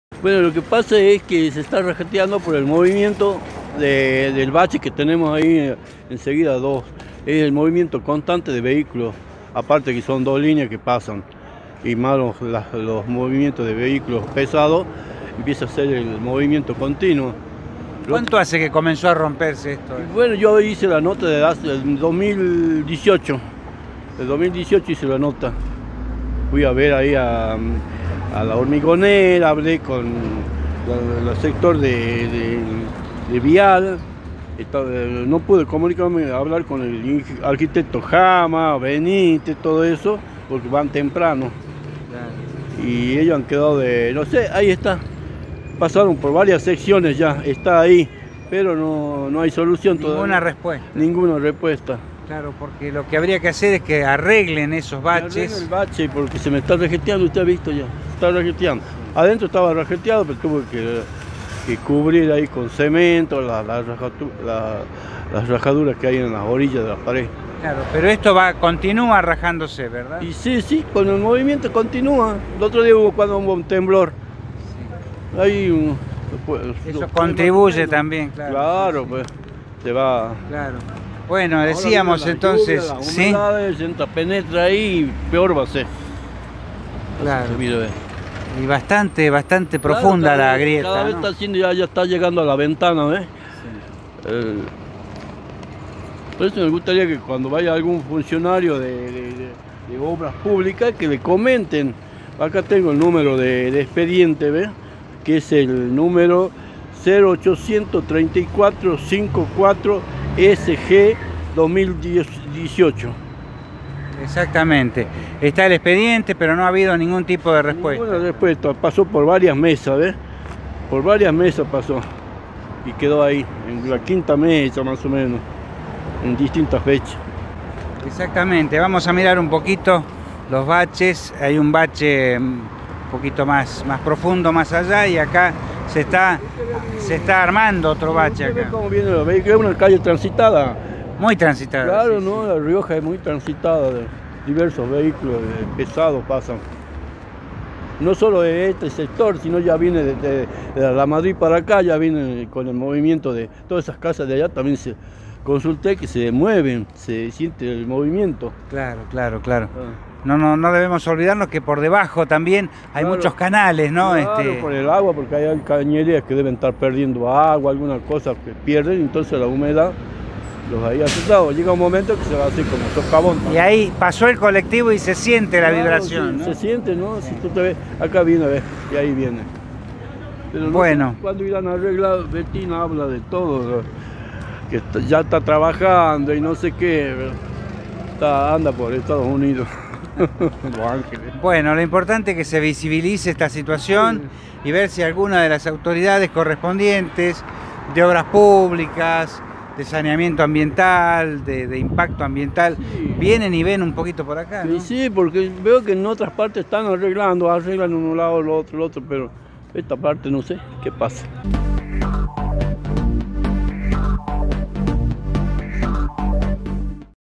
SALTA - Un importante bache provocó rajaduras en una vivienda
Juan Carlos Ávalos - vecino de Calle La Rioja al 1.700